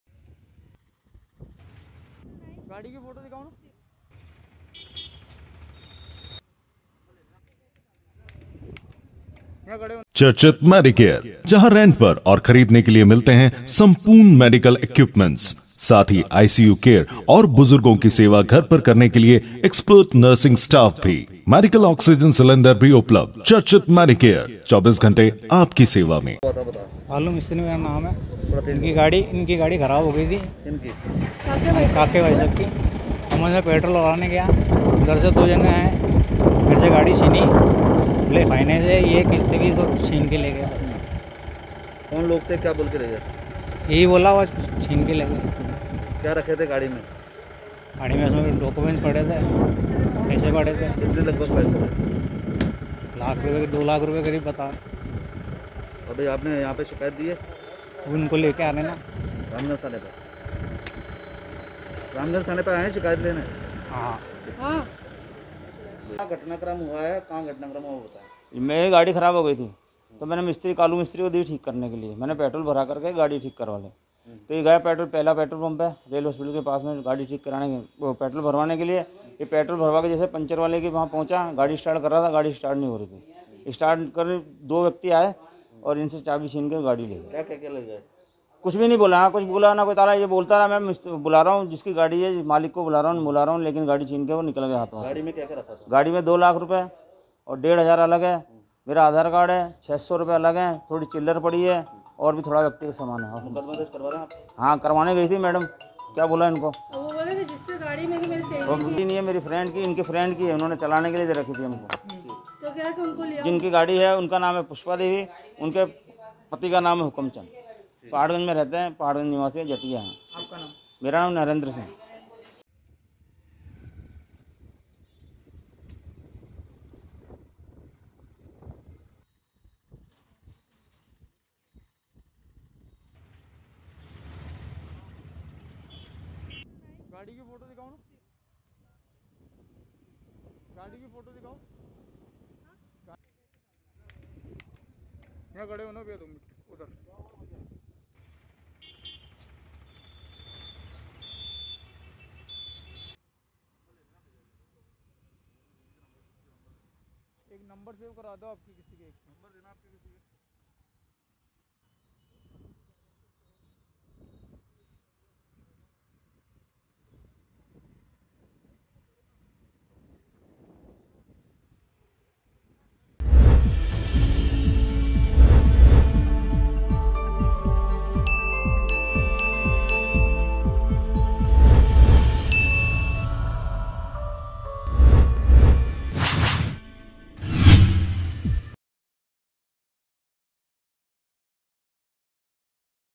Ajmer | रामगंज स्थित रेलवे अस्पताल के सामने मिस्त्री से गाड़ी लेकर भागे दो अज्ञात युवक | MTTV INDIA
रामगंज स्थित रेलवे अस्पताल के सामने मिस्त्री से गाड़ी लेकर भागे दो अज्ञात युवक
फाइनेंस कंपनी के कर्मचारी बताकर की वाहन लूट की वारदात
वाहन मालिक ने बताया गाड़ी में रखे थे 2 लाख रुपए व अन्य दस्तावेज
रामगंज थाना पुलिस कर रही है मामले में जांच